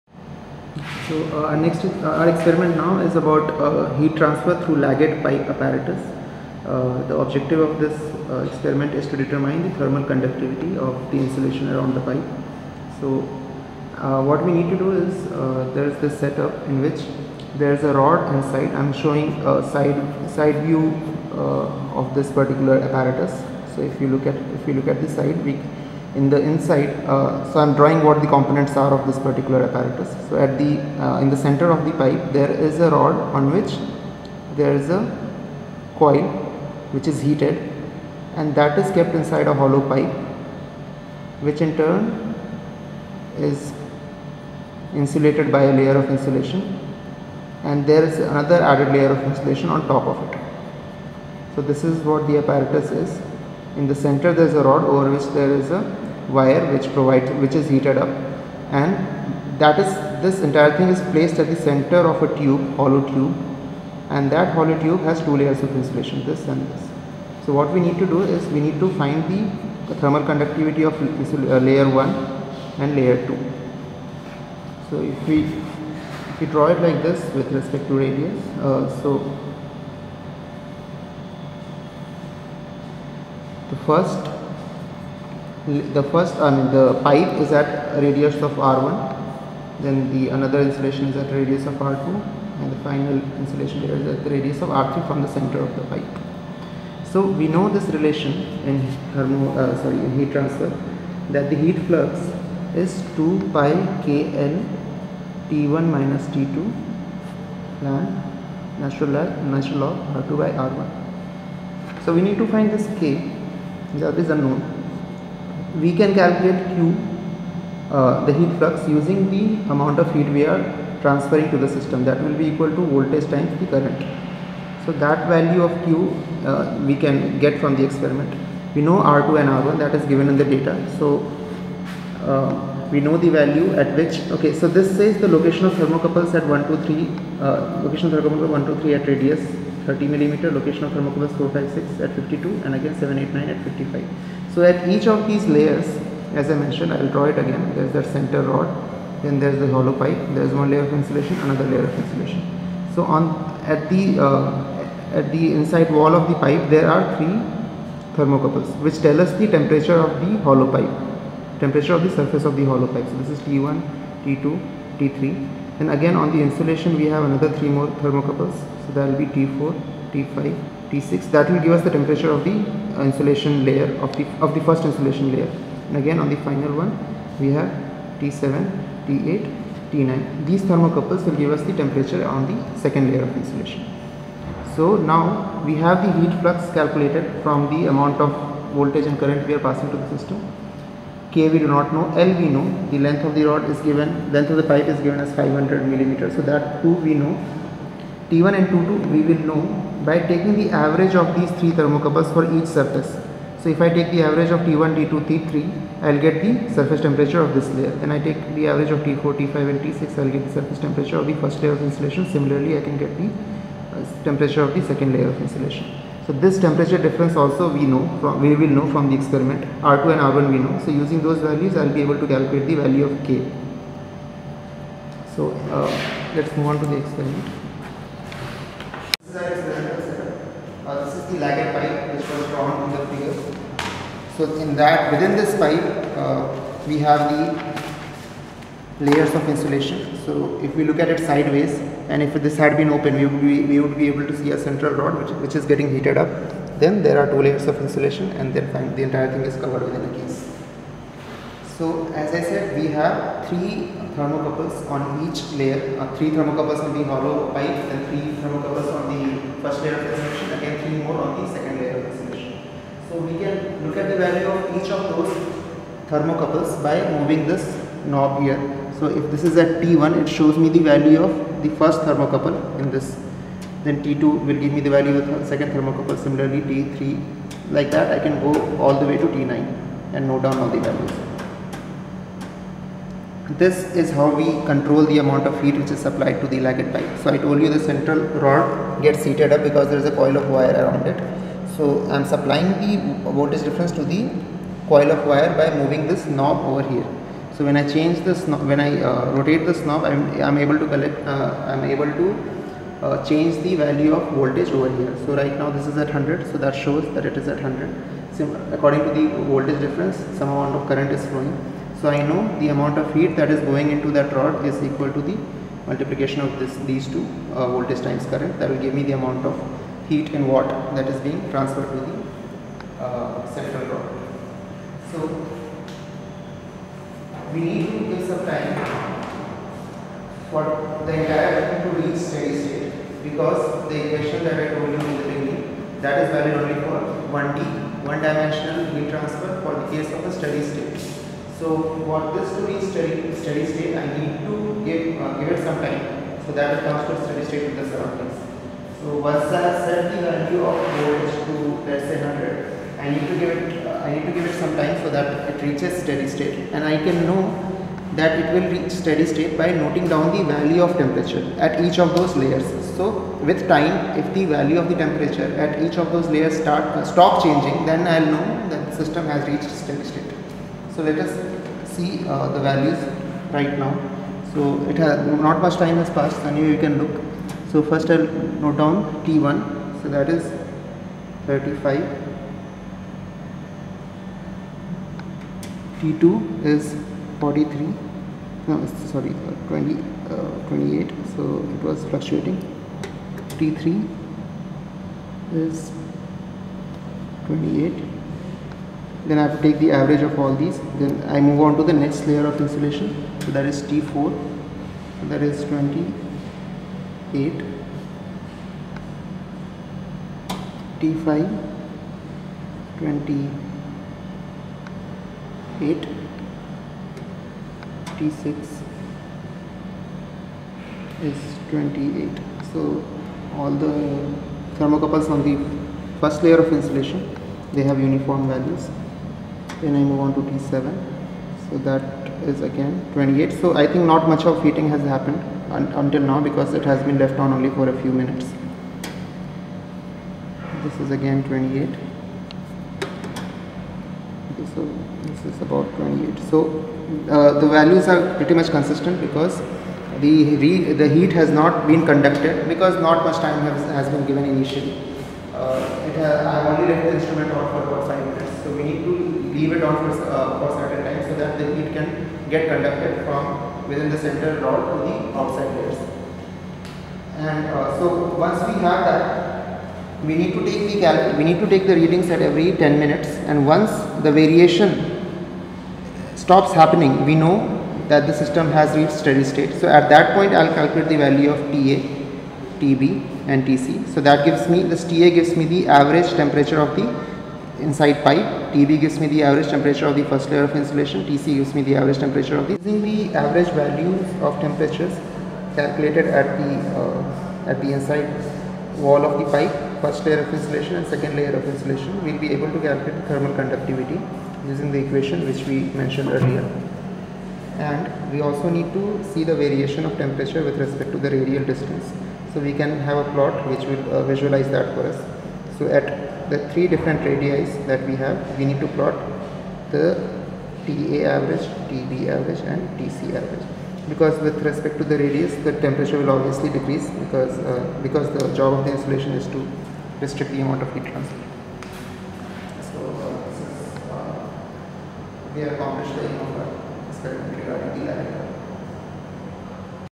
Experiment No: 6 Heat transfer through lagged pipe apparatus.
OBJECTIVE:
Plot the radial temperature distribution in the composite cylinder and determine the thermal conductivity of the pipe insulation
Demonstration by:
                                 DIVYAPRAKASH
                       Assistant Professor (TEQIP)
             Department of Mechanical Engineering
      Jorhat Engineering College, Jorhat. Assam-785007